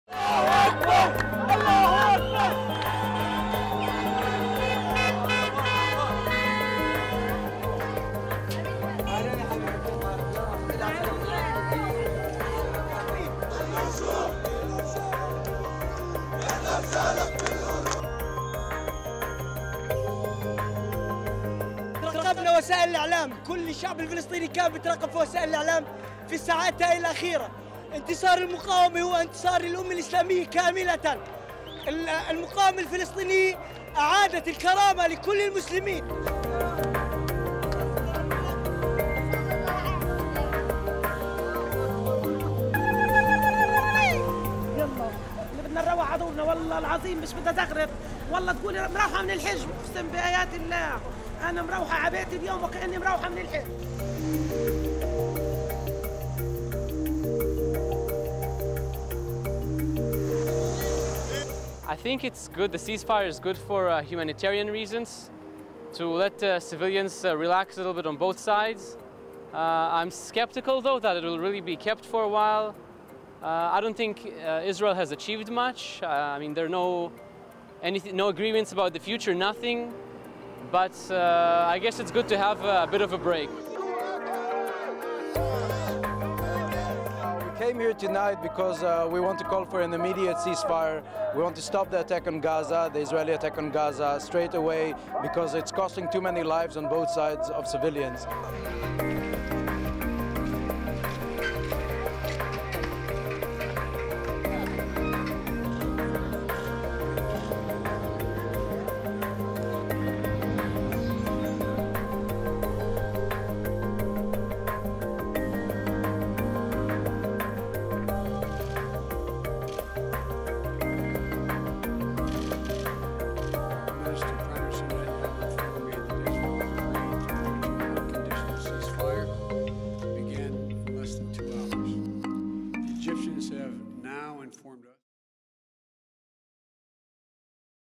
Así celebraron palestinos el inicio de alto el fuego entre Israel y Hamas
Tras 11 días de conflicto, la tregua, negociada por representantes del gobierno egipcio, fue respetada tanto por el gobierno israelí como por el grupo de militantes islamistas.